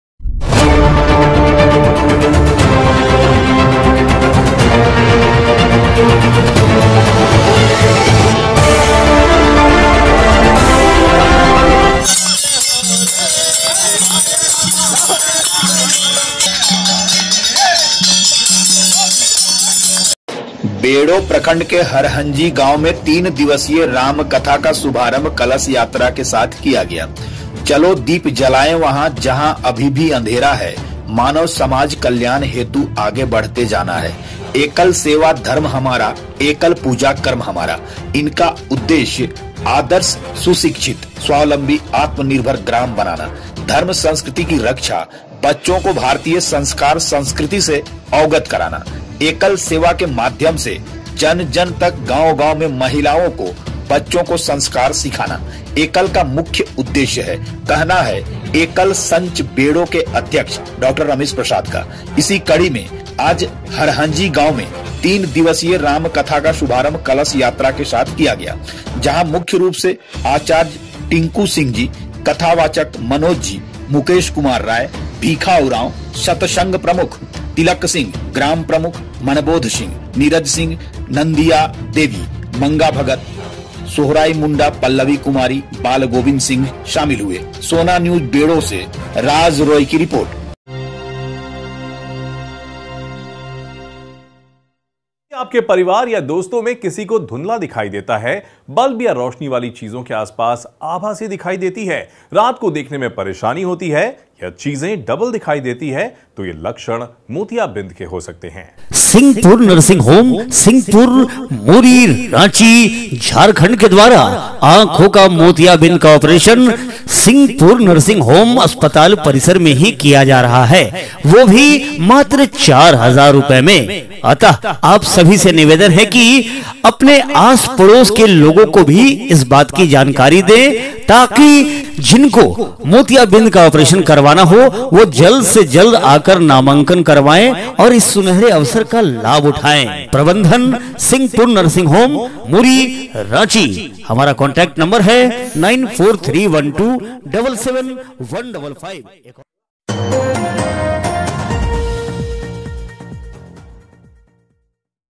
बेड़ो//Ranchi//एकल अभियान के तहत हरहंजी गांव में तीन दिवसीय रामकथा सह कलश यात्रा का हुआ शुभारंभ।
एकल अभियान के तहत हरहंजी गांव में तीन दिवसीय रामकथा सह कलश यात्रा का हुआ शुभारंभ।